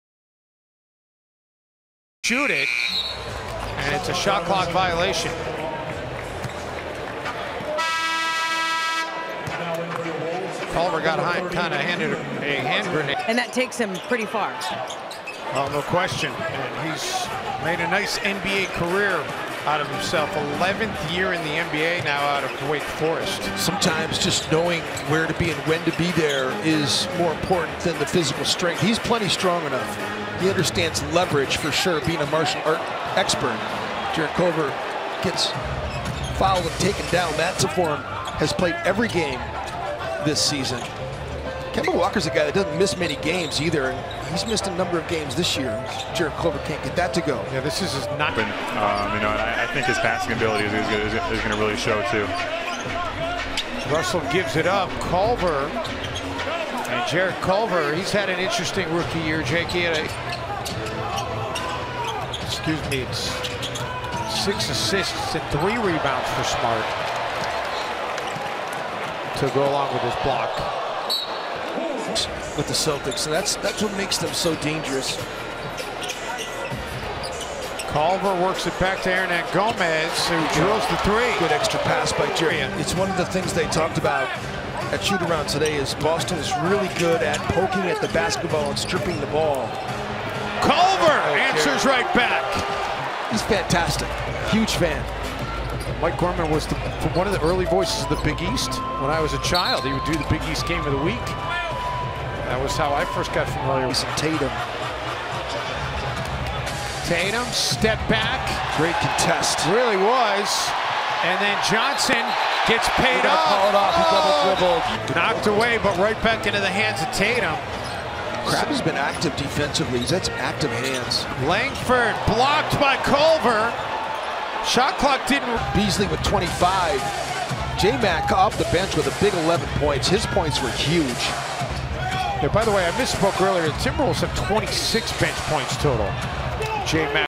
Jarrett Culver Full Play vs Boston Celtics | 02/21/20 | Smart Highlights
Boston Celtics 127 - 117 Minnesota Timberwolves